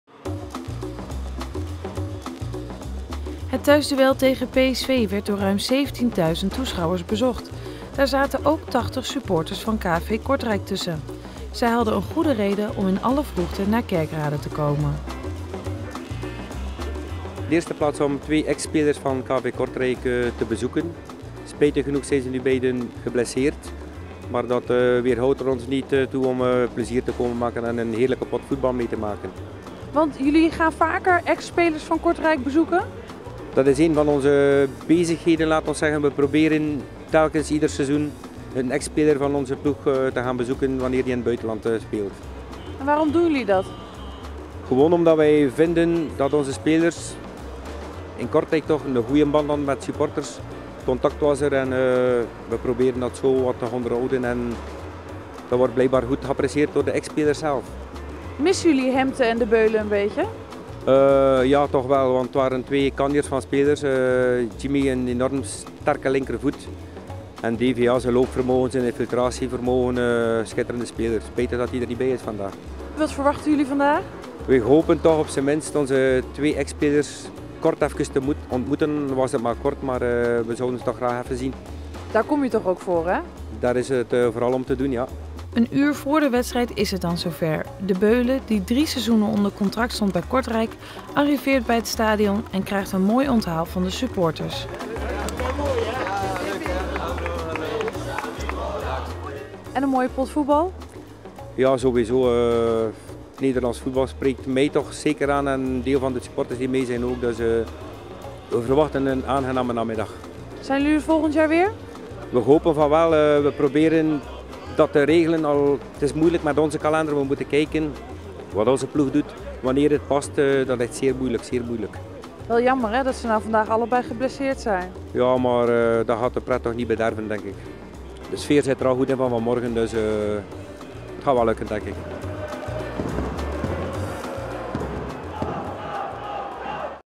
FanClub Bo Braem (KVK) bezoek Roda
bezoek aan Jimmy Hempte en Davy De Beule